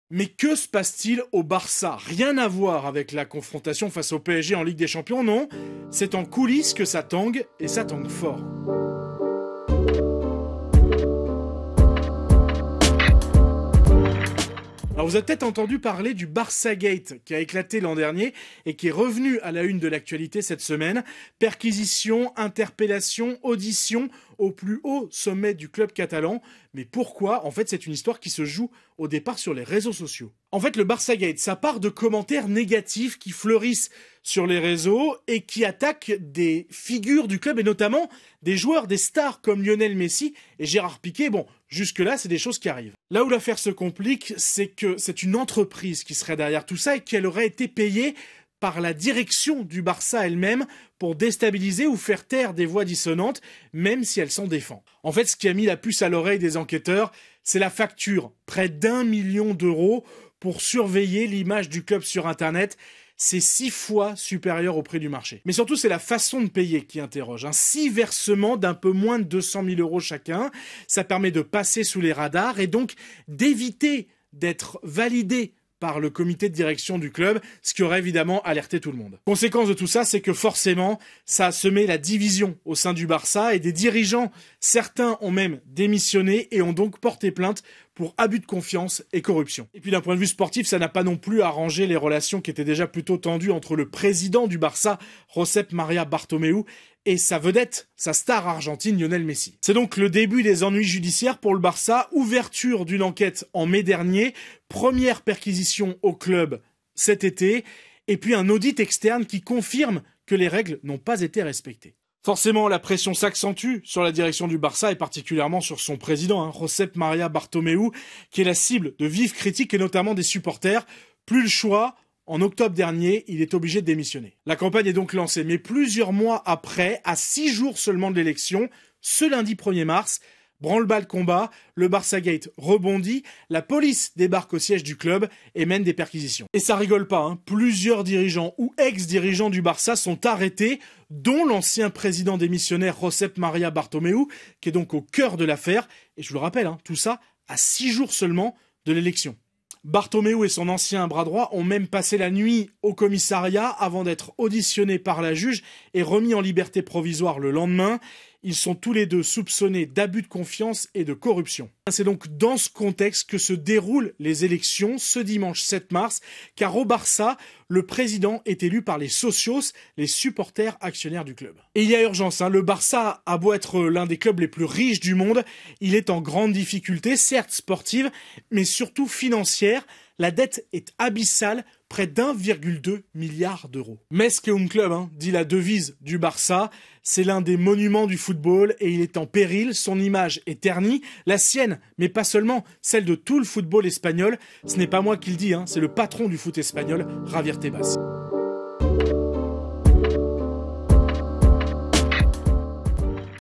Qu'est-ce que le "Barçagate" ?
L'ex-président du FC Barcelone, Josep Maria Bartomeu, rattrapé par la justice, la superstar Lionel Messi qui rêve d'ailleurs, la dette abyssale... Géant du football mondial, le FC Barcelone se débat dans une interminable crise avant l'élection de son nouveau président dimanche 7 mars 2021, puis un choc européen crucial face au Paris-Saint-Germain, en Ligue des champions.
On vous explique ce qu'est le séisme du "Barçagate" qui résument l'année "horribilis" du Barça.